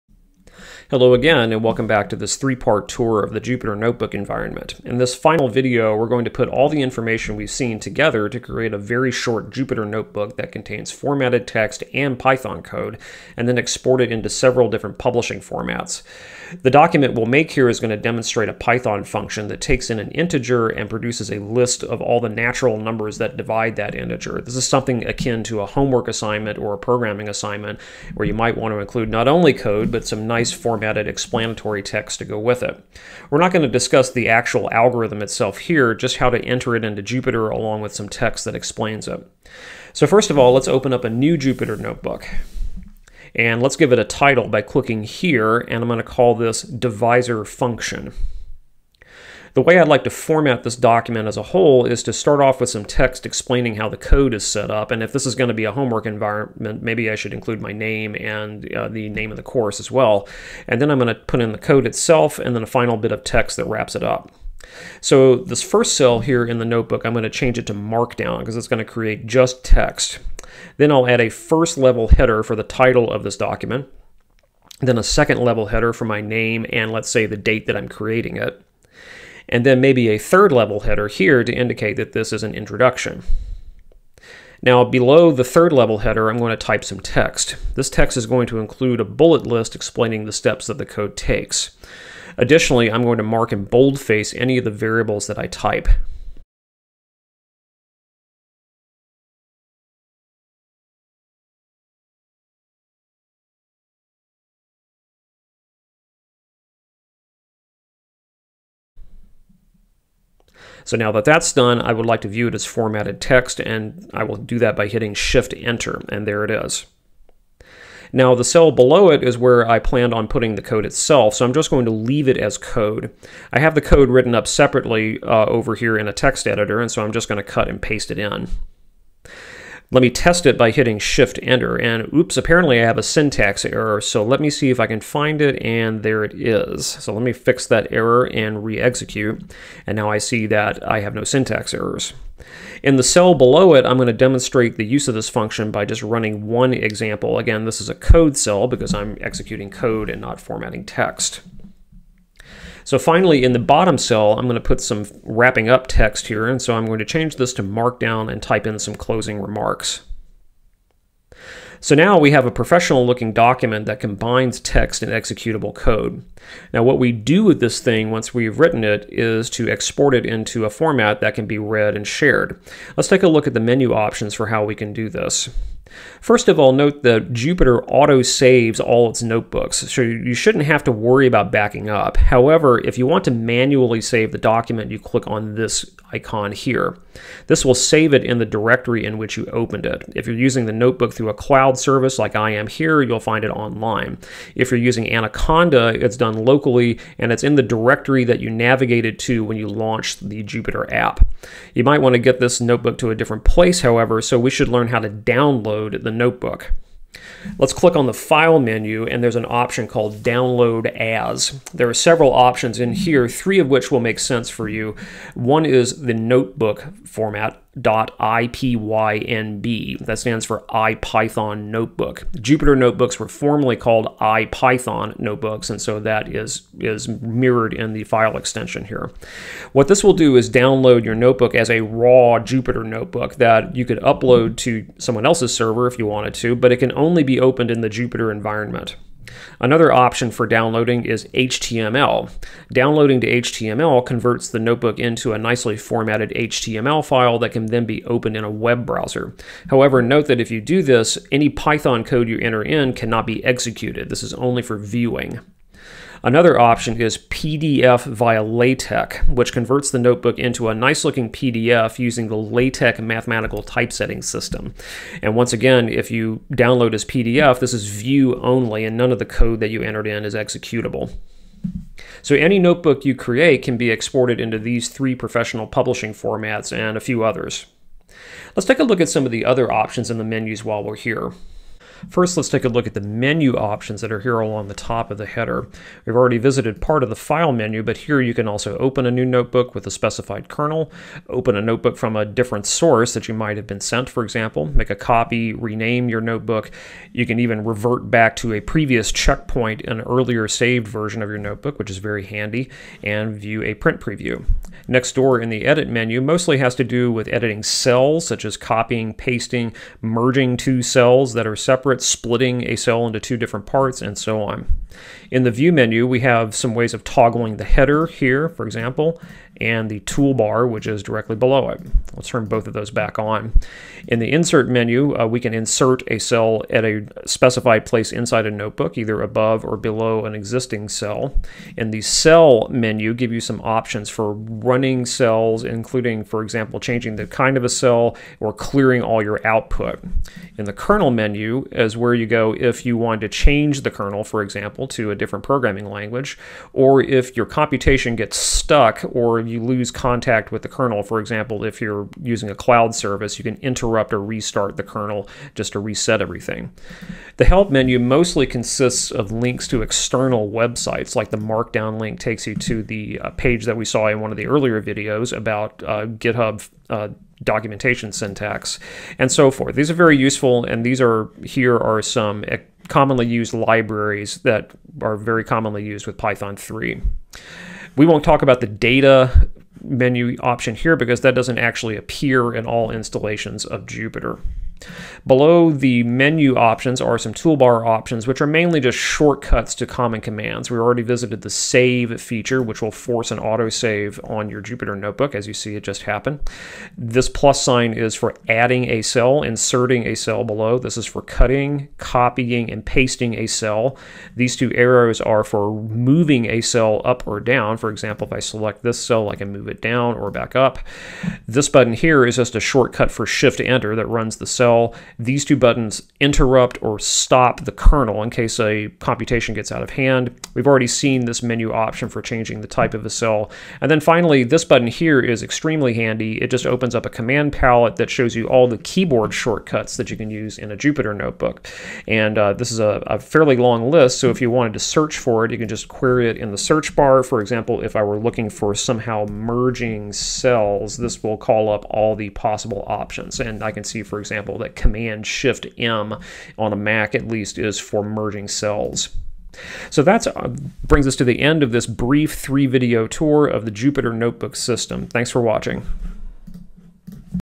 The Jupyter Notebook Environment part 3
Part 3 of a three-part video series on getting started with Jupyter notebooks. This one features a short example of creating a Jupyter notebook from scratch and explores publishing options and the menu bar.